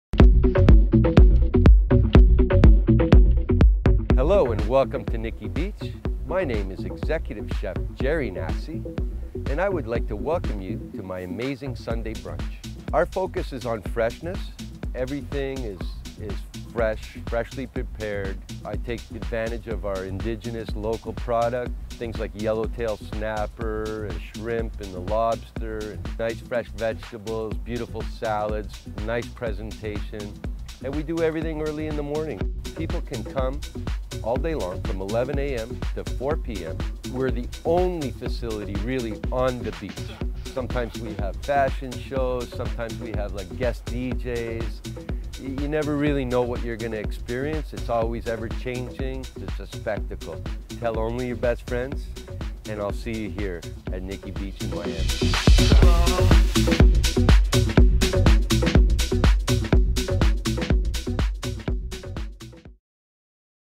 Nikki Beach, Miami - Sunday Brunch - Unravel Travel TV
Nikki Beach in Miami is the hidden jewel of South Beach, located at One Ocean Drive along the beautiful Atlantic Ocean amid swaying palms trees and warm sunny breezes. Nikki Beach Miami has established itself as the landmark nightclub and venue for ultimate private parties, large celebrations, and always lives up to its reputation as a South Beach Miami party playground for jet setters, celebrities, VIPs, guests and visitors alike.
Nikki Beach Club in Miami is comprised of a large oceanfront complex featuring an outdoor beach club, an award winning restaurant and a sexy South Beach nightclub favorite. Enjoy full food and VIP bottle service in our fabulous cabanas and lounge beds during the day while soaking up the South Beach sun. *Dress code at Nikki Beach includes casual day wear and beach chic attire.

The South Beach flagship property's menu is influenced by Nikki Beach properties around the world from St. Tropez to Marbella, Koh Samui and beyond. Executive Chef Gerry Gnassi's inspiration stems from the fresh seafood and flavors of the South of France, spices of East Asia and South Beach's laissez- faire attitude. Nikki Beach Miami creates the perfect blend of flavors bound to please every gourmand's palate.

The Nikki Beach restaurant is one of the only gourmet dining destinations in South Beach to actually offer a beach and oceanfront restaurant dining experience. Nikki Beach offers an eclectic menu ranging from wood burning oven pizzas, delicious salads, creative sushi rolls, innovative seafood entrees, freshwhole lobster and mouthwatering Chateaubriand. The perfect mix of jet-set style and South Beach's exciting and sexy vibe, Nikki Beach is a not-to-be-missed destination not only for dining but also enjoying a truly unique South Beach landmark.